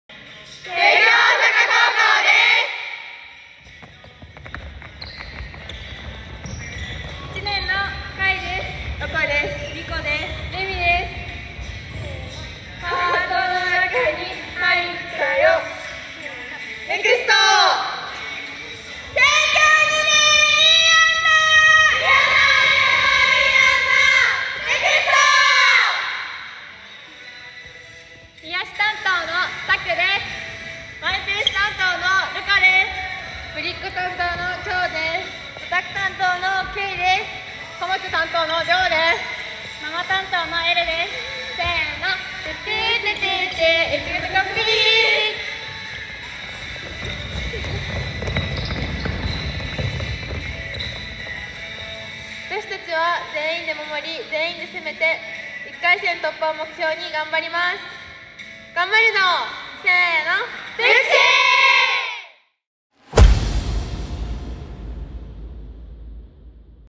ウインターカップ2020 帝京安積（女子・福島県代表）紹介ムービー
SoftBank ウインターカップ2020
2020年、唯一の高校日本一へ。

SoftBank ウインターカップ2020への出場校は男女60の計120校。これらの動画は、各都道府県代表校の意気込みや注目してもらいたいポイントなどを紹介するムービーです。日頃から応援してくださる学校関係者や地域の方、バスケットボールファンのみなさまなど多くの方に知っていただきたくチームで考えて作りました。ぜひ、私たちのことを知っていただき、熱い応援をお願いします！

★SoftBank ウインターカップ2020 大会特設サイト